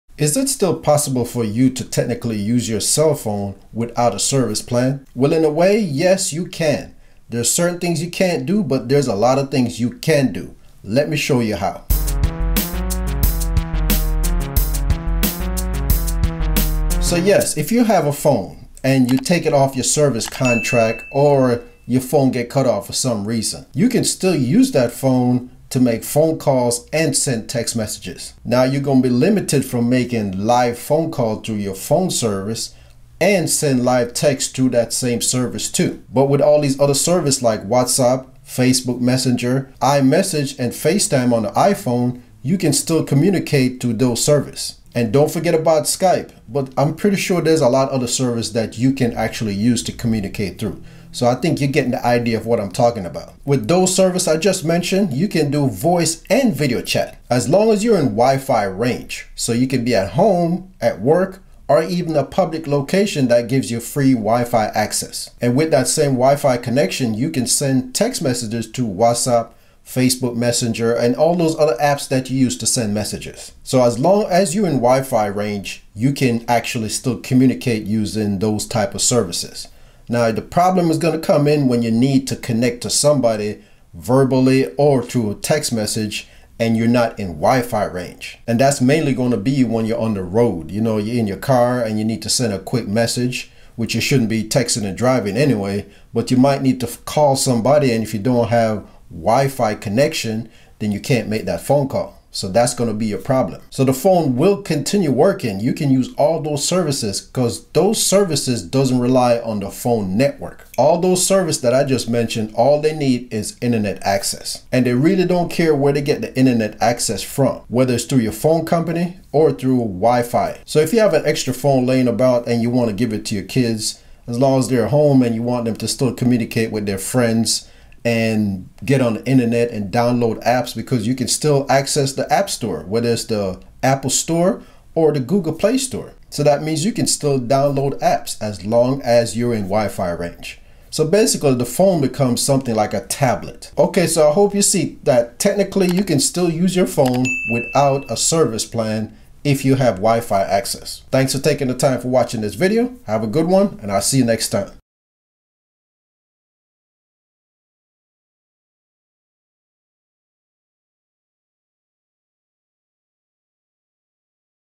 Use Your Phone Without A Service Plan To Make Calls & Send Text Messages.
Can you still use your phone without a service plan?  Well, technically it's possible to send text & make voice calls without a service plan from the phone company, let me tell how that's possible.